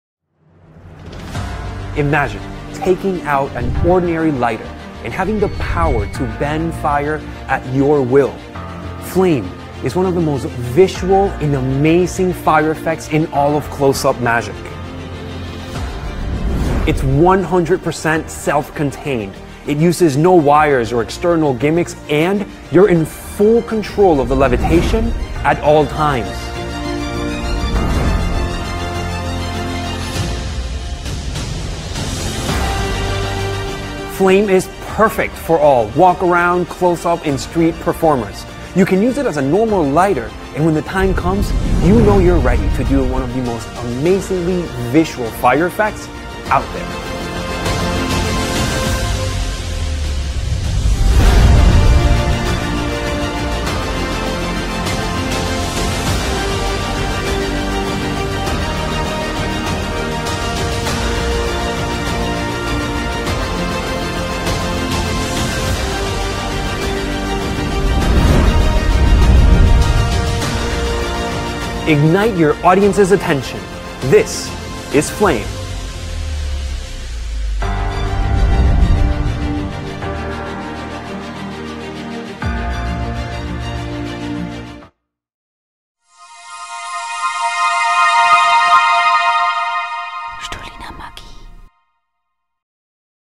Flame by Murphys Magic
Dieser Artikel ist ab sofort über unseren Online-Shop erhältlich:

Sie als Profi wissen es schon längst. Wenn Sie die Aufmerksamkeit Ihres Publikums bekommen möchten, setzen Sie auf die Macht des Feuers.

FLAME ist bei weitem einer der visuellsten CloseUp-Effekte mit Feuer!

Sie geben ein gewöhliches Feuerzeug zum Überprüfen! Sie entzünden es, und dann, plötzlich und unerwartet steigt die Flamme auf Ihr Kommando nach oben, oder wieder
zurück zum Feuerzeug. Ganz wie Sie wünschen. Stellen Sie sich vor, dass Sie etwas Pyropaiper weit oberhalb Ihres Feuerzeuges halten. Im normalen Leben, so sagen Sie, müssten Sie jetzt mit dem Pyropapier zum Feuerzeug gehen. Sie aber haben die Möglichkeit, durch übersinnliche Kräfte, die Flamme nach oben zum Pyropapier steigen zu lassen. Im selben Augenblick steigt die Flamme empor, und entzündet Ihr Pyropapier.

Flame ist zu 100% selbsterklärend. Schauen Sie sich einmal das Anleitungsvideo an, und Sie wissen sofort was zu tun ist. Sie haben immer die volle Kontrolle über die Flamme.

Stecken Sie Flame in Ihre Tasche, und Sie werden immer ein starkes Stück Magie bei sich tragen. Damit Sie sofort starten können, legen wir Ihnen einen Bogen Pyropapier gratis bei. Nachschub finden Sie hier: Pyropapier

Flame ist ein Muss für jeden CloseUp Performer, Walkarround-Künstler, oder Straßenkünstler!

Flame ist:

100% selbsterklärend
100% unter Ihrer Kontrolle
Kabellos
ohne exterme Gimmicks
ein normales Feuerzeug
erstaunlich Visuell
Sie erhalten das Feuerzeug, einen Link zu einem englischsprachigen Download, einen Link zu einer deutschsprachigen Videokurzanleitung, 1 Bogen Pyroparier!

Abgabe nur an volljährige Personen.